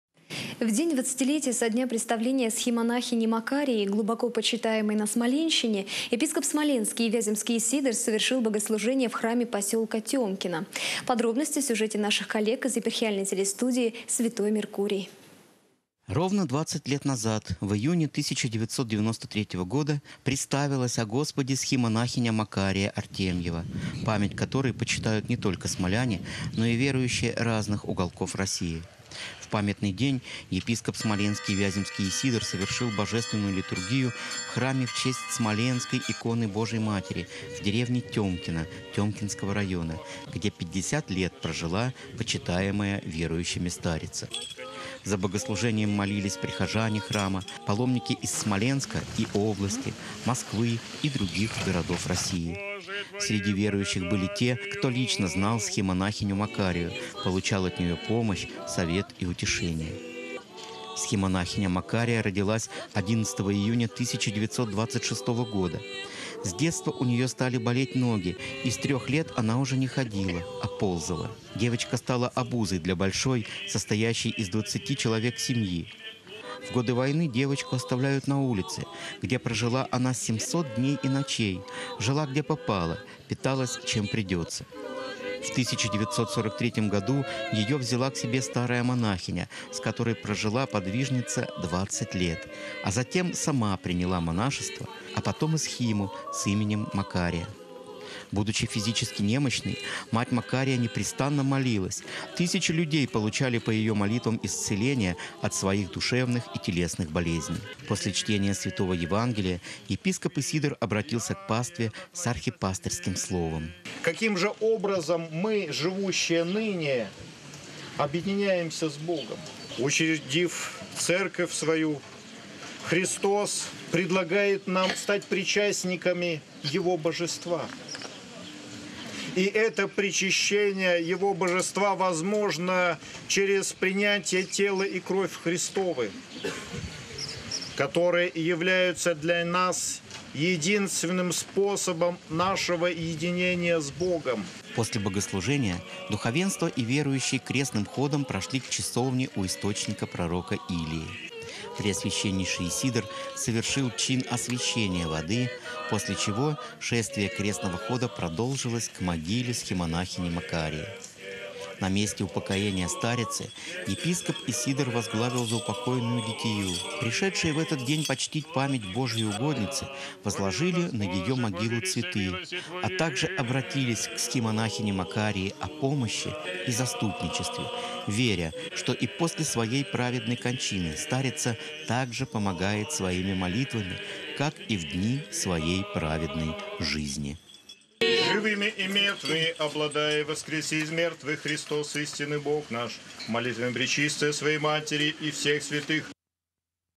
В день 20-летия со дня преставления схимонахини
В день 20-летия со дня преставления схимонахини Макарии, глубоко почитаемой на Смоленщине, епископ Смоленский и Вяземский Исидор совершил богослужение в храме поселка Тёмкино.